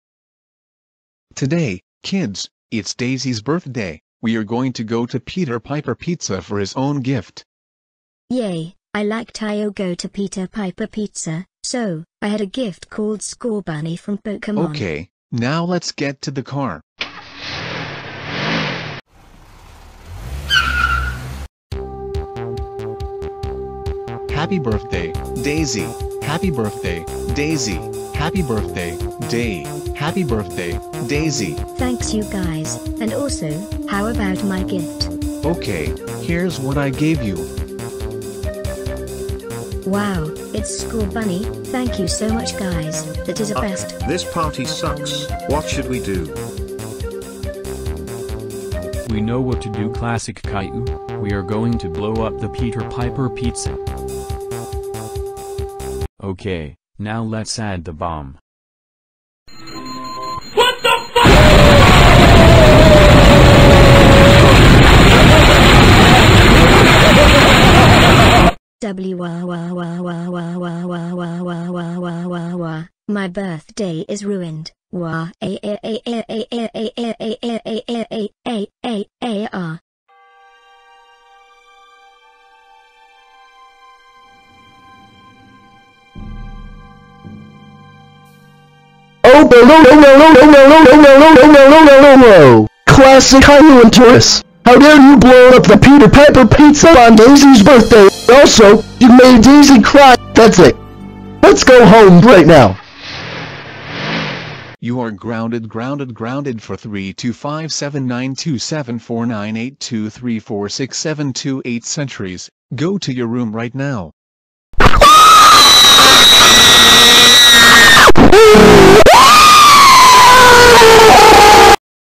Classic Caillou and Toris Blows Up the Peter Piper Pizza on Daisy's Birthday/Grounded
Classic Caillou and toris blows up the peter piper pizza for daisy's birthday who got Scotbunny, nothing else about these 2 troublemakers.